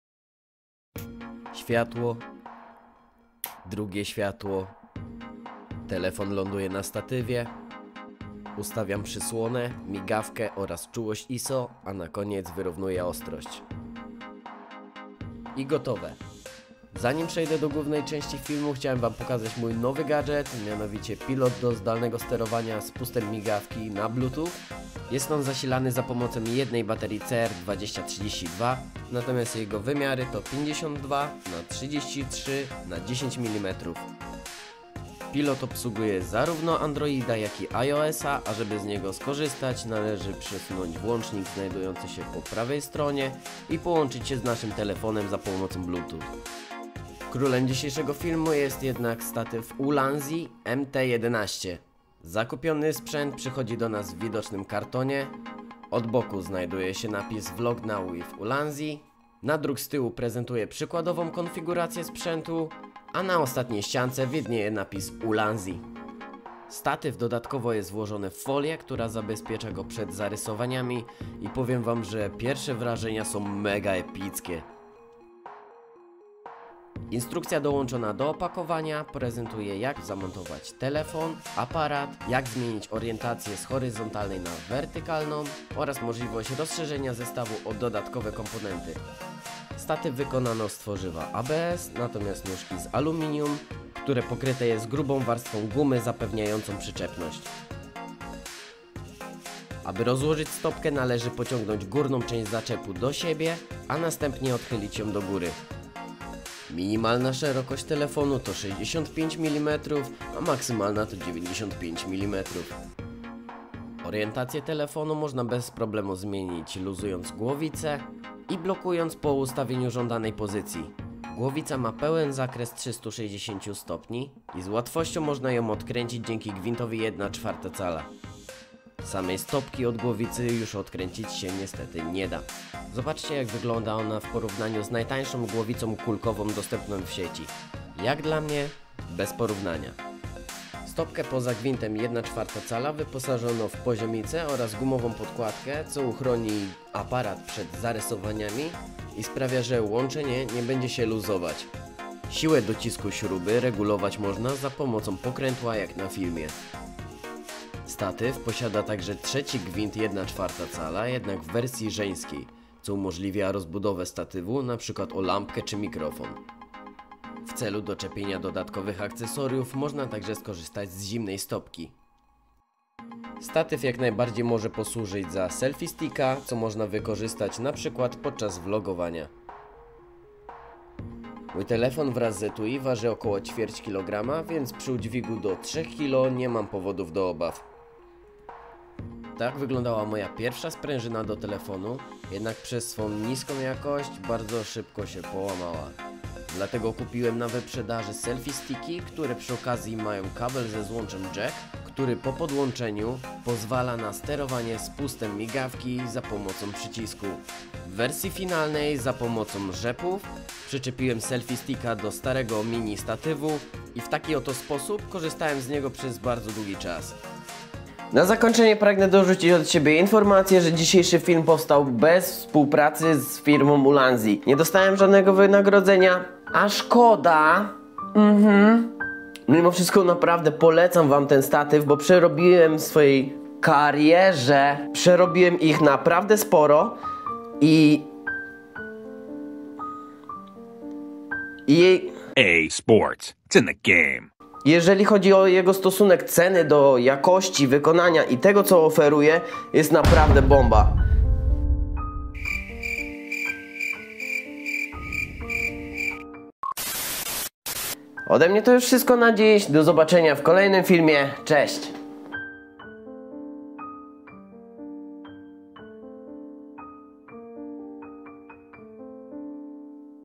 TO NAJLEPSZY TANI STATYW do TELEFONU!! 📱😮 LEPSZY niż GORILLAPOD? | MUZYKANAŁ #4
Wybór statywu do telefonu to wbrew pozorom nie taka prosta sprawa. W gąszczu modeli dostępnych na rynku idzie się łatwo pogubić, a nie sztuką jest przepłacić za badziew, który po kilku użyciach będzie nadawać się do kosza. 😢😢

Po KILKU DNIACH researchu w końcu udało mi się podjąć dobrą decyzję, której nie raczej szybko nie będę żałować. Zastanawiałem się długo między statywem Ulanzi MT-11, a słynnymi Gorillapod. Różnica w cenie i obejrzenie testu wykonanego przez Michaela Tanga (czytaj poniżej) przeważyły jednak przy moim wyborze. 💰💰 Zachęcam również gorąco do zobaczenia filmu Ziemniaka, który również przygotował materiał na temat statywów (jednak już nie do telefonów).

- Dr. Michael Tang PT, DPT, CSCS - Joby Gorillapod or Ulanzi Camera Stand? 1K, 3K, & MT-11- Review and Comparison
- [FAQ Ziemniak] - Jakie statywy mam u siebie?

Spis odcinka:
0:00 - 0:15 - Wstęp
0:16 - 0:46 - Pilot zdalnego spustu migawki (Remote Shutter)
0:47 - 1:05 - Statyw Ulanzi MT-11 (opakowanie)
1:06 - 1:16 - Unboxing
1:17 - 1:28 - Przegląd instrukcji
1:29 - 1:38 - Wykorzystane materiały
1:39 - 2:00 - Montaż telefonu na statywie
2:01 - 2:19 - Głowica kulkowa
2:20 - 2:36 - Montaż aparatu do stopki
2:37 - 2:52 - Montaż dodatkowych akcesoriów
2:53 - 3:08 - Zastosowanie jako selfie stick
3:09 - 3:37 - Historia moich statywów
3:38 - 4:31 - Mowa końcowa